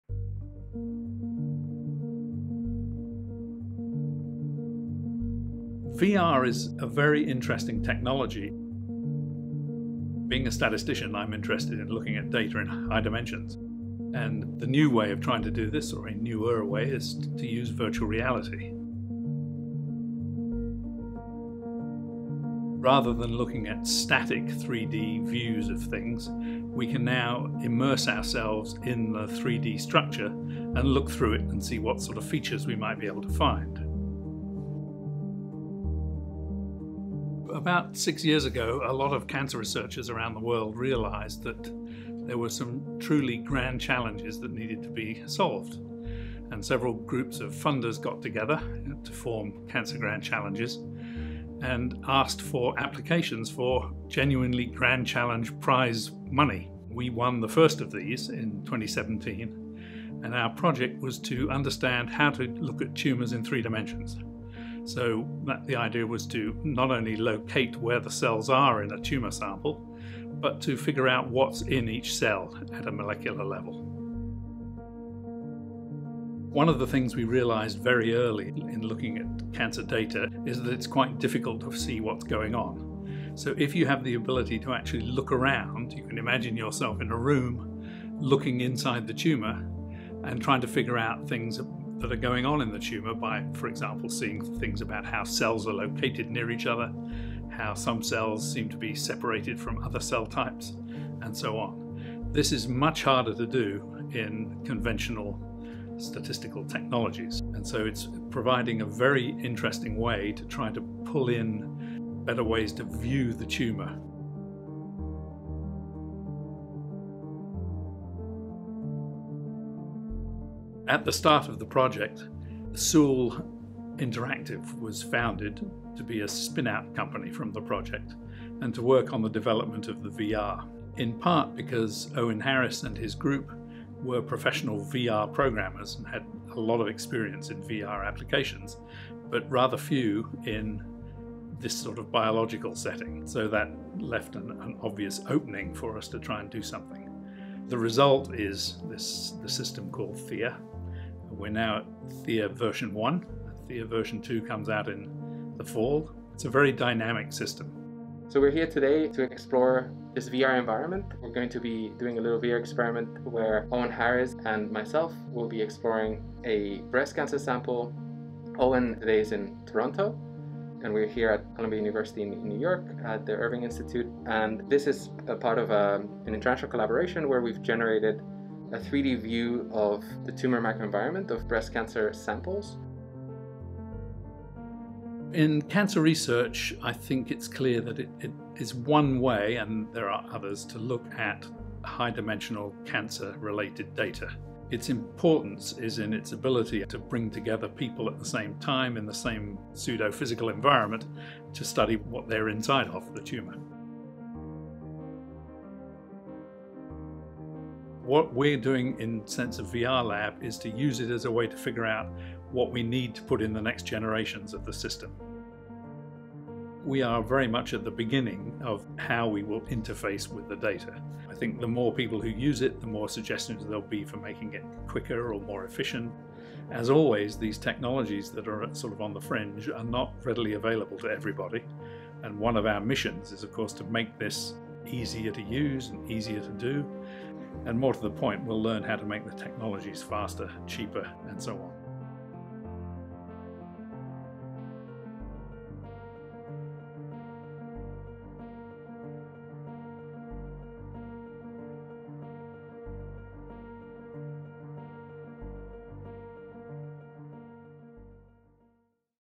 Why Virtual Reality Is the Future for Columbia Cancer Research
“Rather than looking at static 3D views of things we can immerse ourselves in the 3D structure and look through it and see what kinds of features we might be able to find,” says Simon Tavaré, director of the Irving Institute for Cancer Dynamics and professor of statistics and biological sciences. Visualizing tumors in three dimensions through virtual reality technology, Tavaré and his colleagues are able to extract new insights from the data.

With VR, Tavaré can immerse himself in cell structures and locate where tumors are in a sample.

Now, Tavaré is developing a VR lab at Columbia University where he says he’s just beginning to interface with the data.

 (This video was produced following strict social distancing and public health guidance)